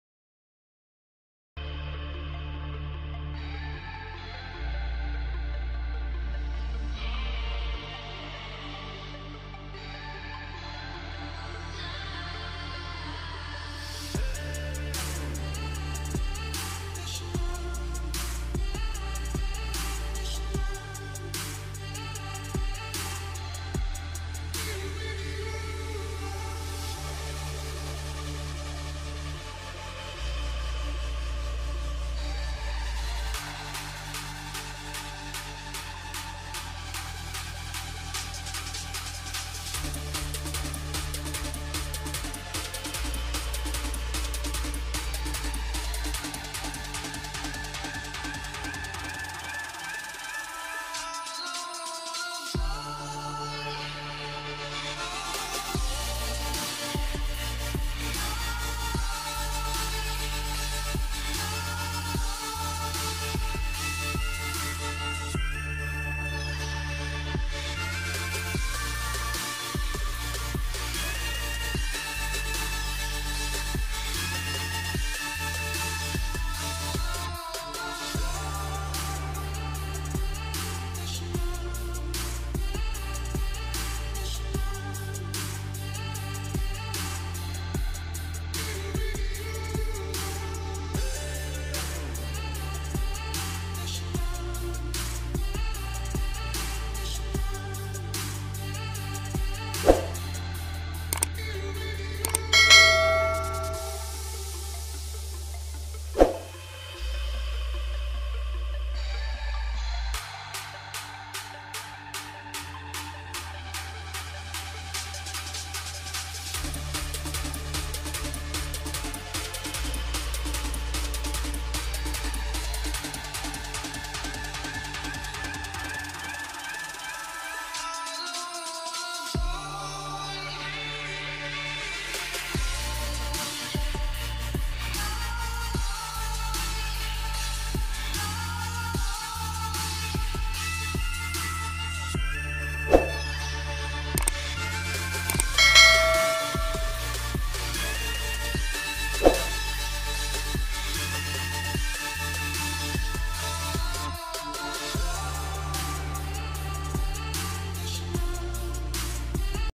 Tecno Camon 20 Hard Reset | Tecno (CK6) Pattern Lock Remove Without Pc | Password Forgot
1- All Infinix FRP\Google Account Bypass Without Pc| Android 12,13 | Apps Not Open | Android SetUp Not Open |

REGARDS :-
BK SMART POINT